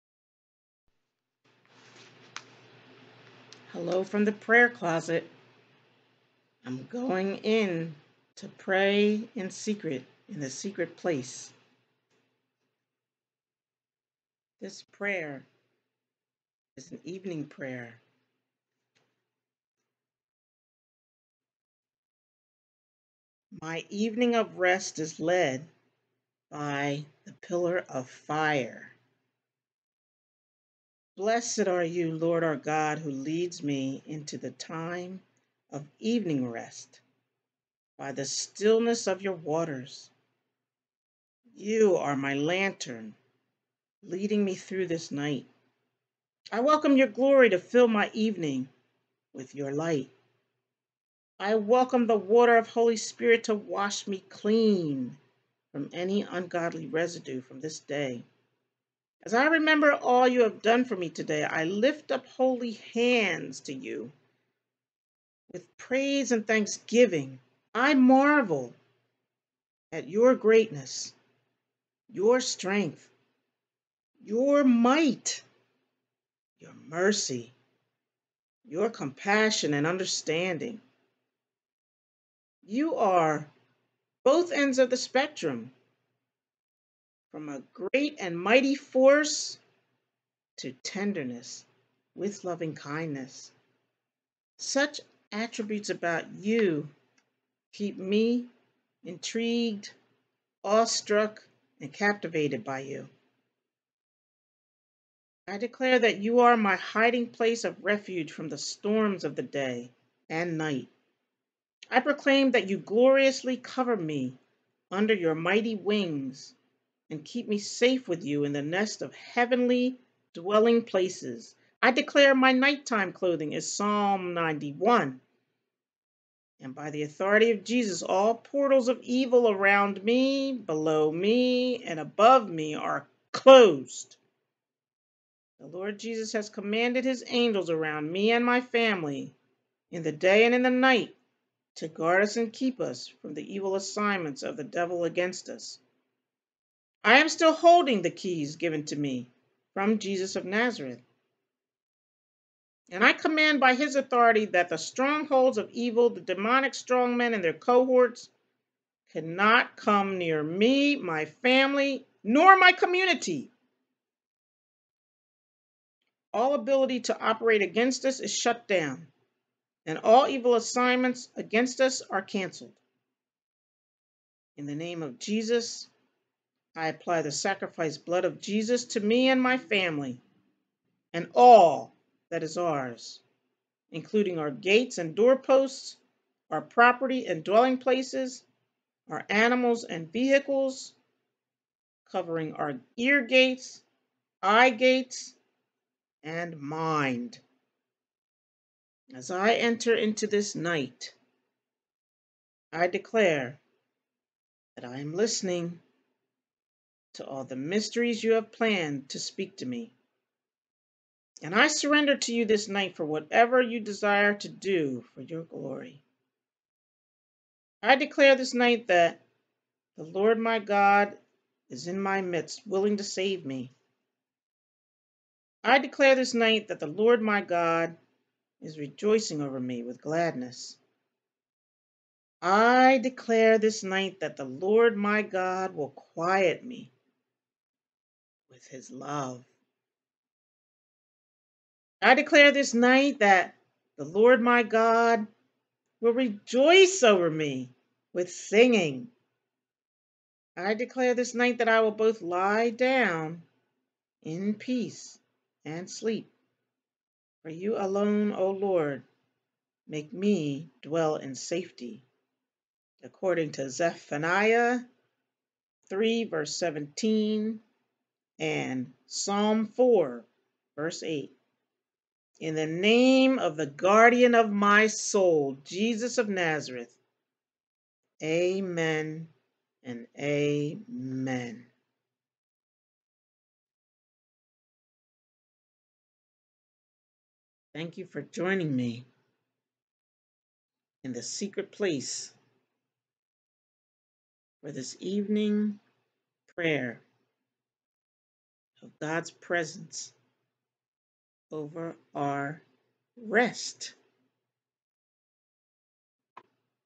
PRAY FOR EVENING REST AND PROTECTION
Pray for God to preserve rest and refreshing for you and your family and keep you safe in His care.